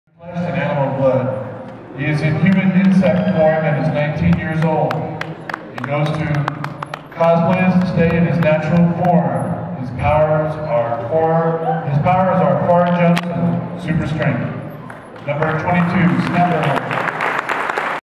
Alien reaching out for abductions during costume contest
Alien reaching out towards the crowd for abductions during the costume contest at the UFO festival.